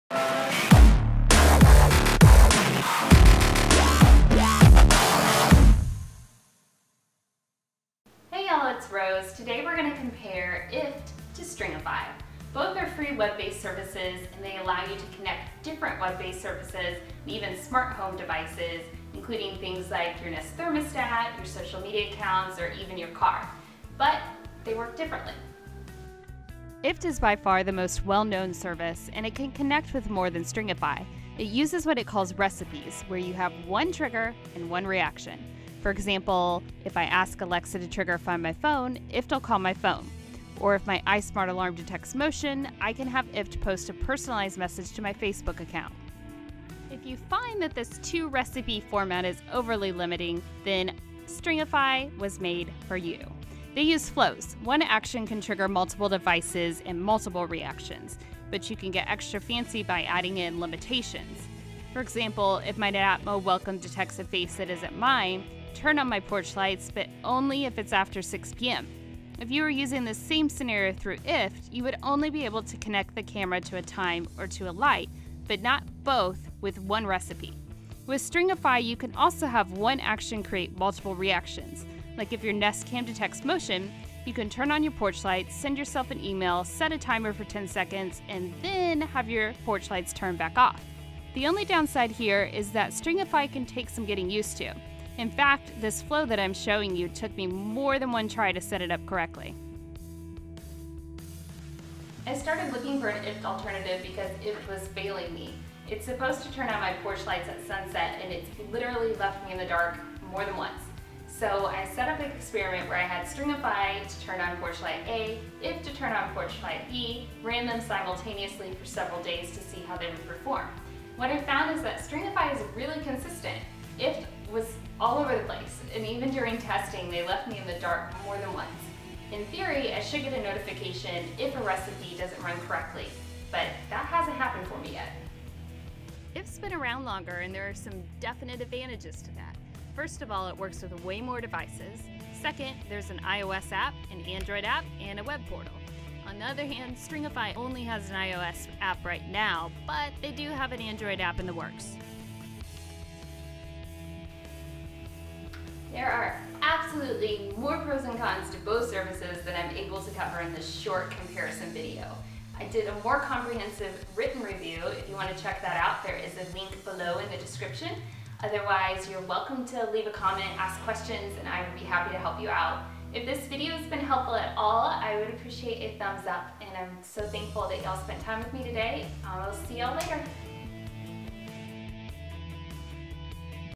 IFTTT vs Stringify - Hands-on Comparison
Stringify is an IFTTT alternative that allows you to create more complex commands for your connected devices. Video includes a comparison of both apps = rules vs flows. For a more comprehensive comparison check this out